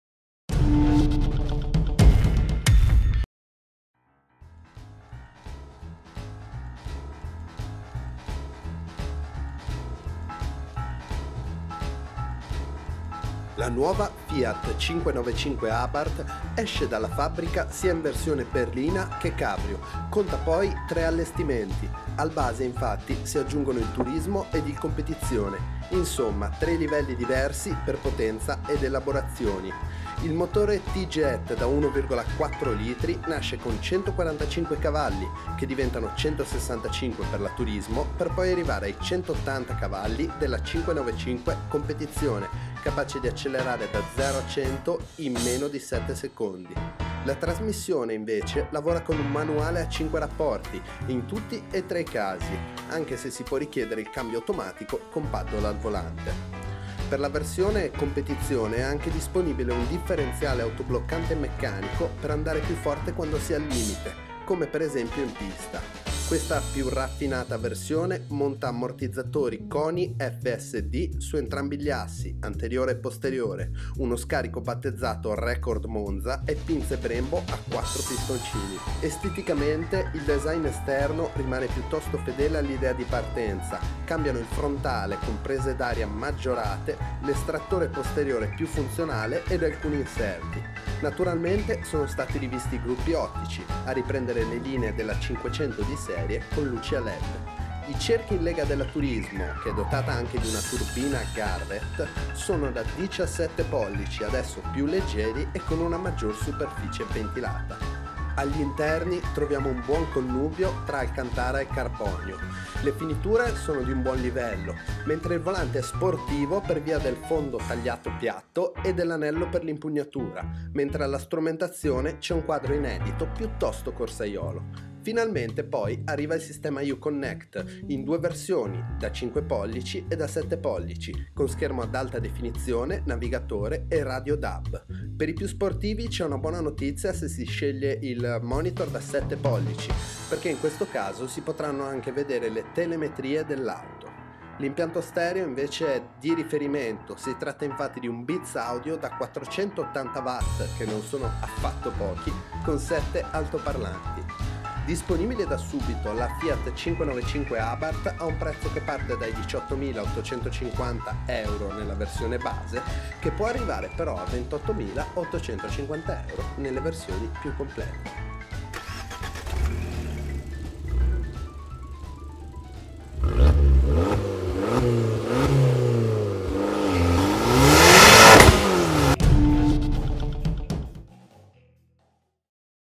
Gamma Abarth 595 2016: Allestimenti, potenza e prezzi
La nuova Gamma Abarth 595 si rinnova per il 2016 con tre diversi allestimenti: 595, Turismo e Competizione. Le potenze vanno da 145CV a 180CV ed un sound coinvolgente: LEGGI QUI TUTTO L'ARTICOLO